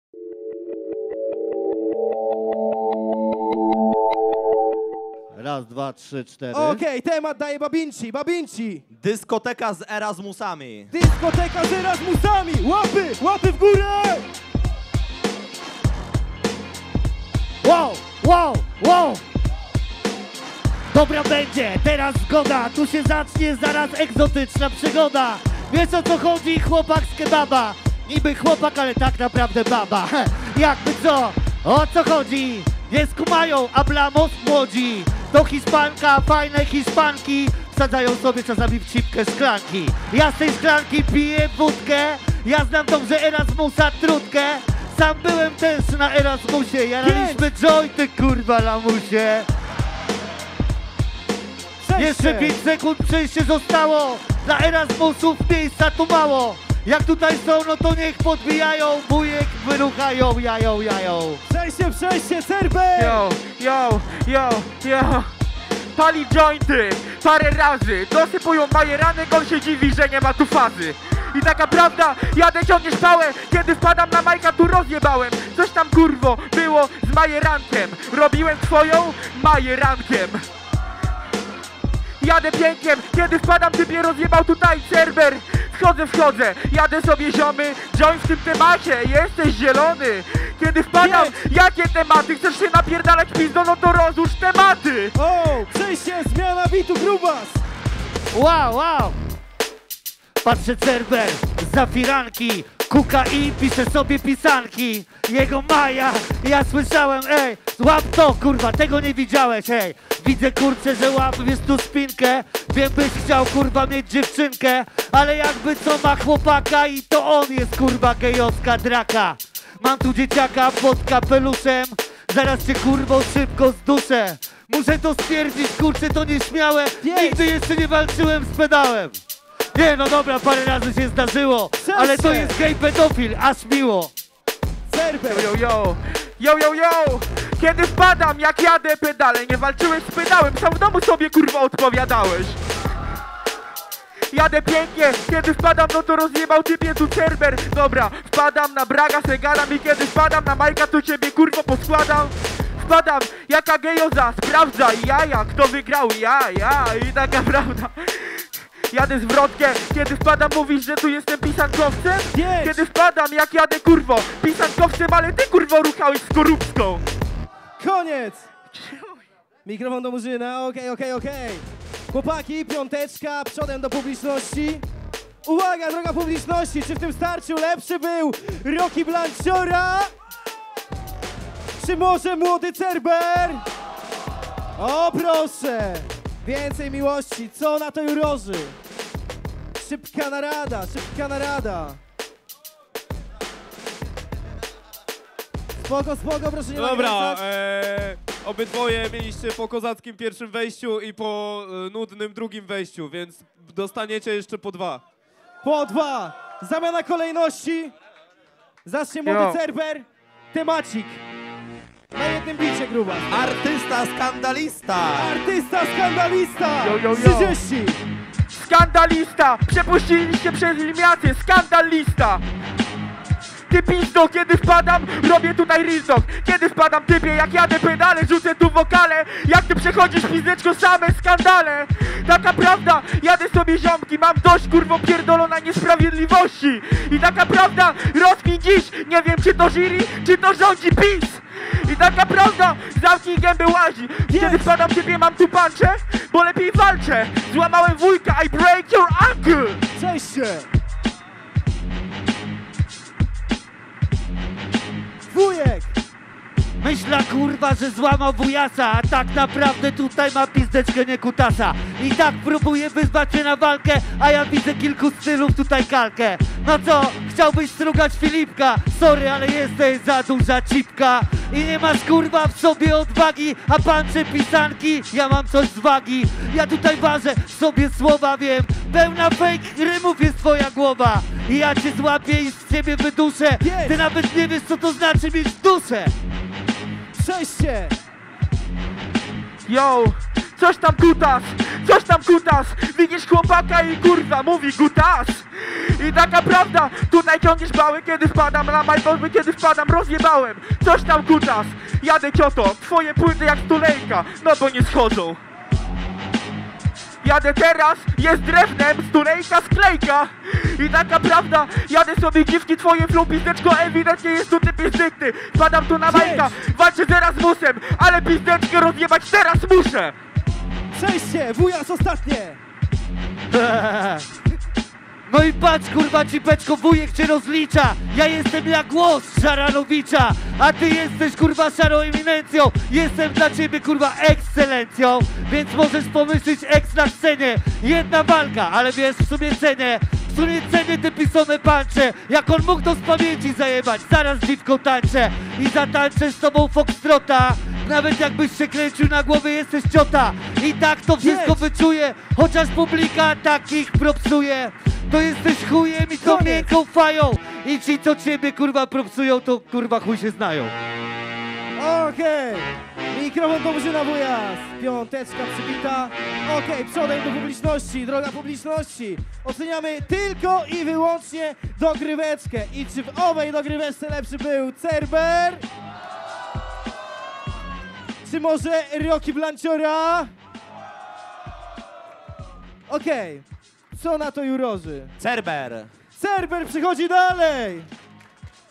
WSZ 🆚 Cerber 🎤 WBW 2019 Warszawa (freestyle rap battle)
freestyle: WSZ vs CERBER
prowadzenie: MURZYN
dj: GRUBAZ
jury: BABINCI, EDZIO

WBW 2019 🎤 Warszawa 🎤 Freestyle Battle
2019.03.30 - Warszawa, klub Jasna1